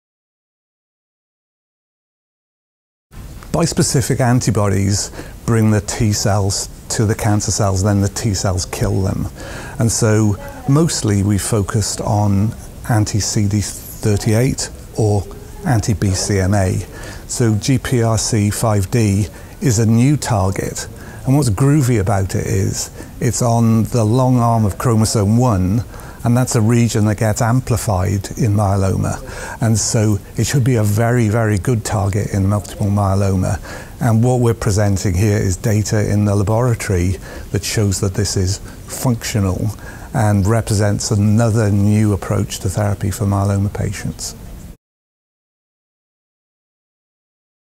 Myeloma: the groovy new GPRC5D
Gareth Morgan, MD, PhD, FRCP, FRCPath, NYU Langone Health, New York, NY, tells us about the GPRC5D receptor located on the long arm of chromosome 1, which becomes amplified in multiple myeloma. Speaking a from the 24th Congress of the European Hematology Association (EHA) 2019, held in Amsterdam, Netherlands, Prof. Morgan discusses lab data showing this target to be functional, representing another approach to therapy for myeloma patients.